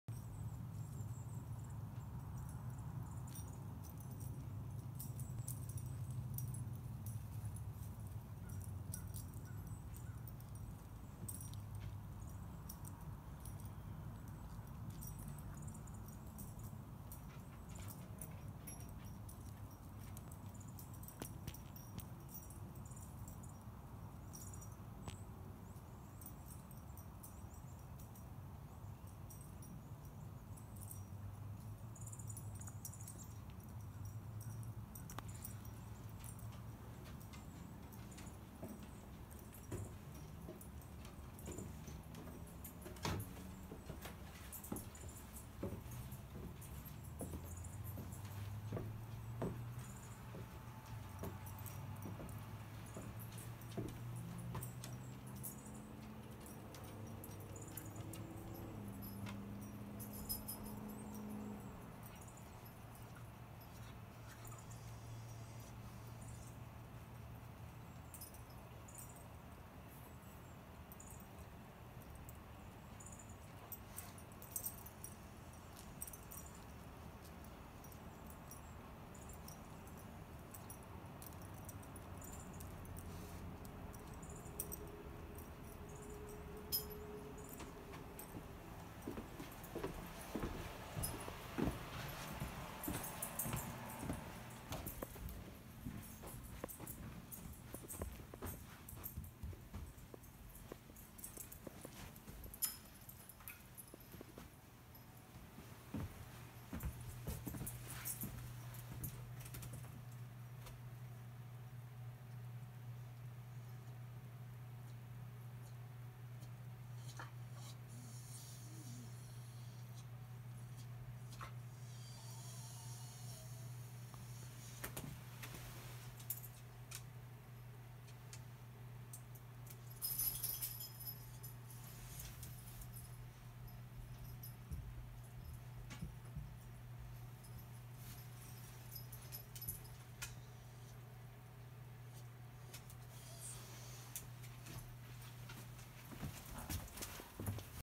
Mimi’s Daily Stroll
Most every day, Mimi our basset makes a big circle down the hill from our house to the garage/apt, then back up the driveway to the front door. This captures the second half of her trip over Thanksgiving 2017.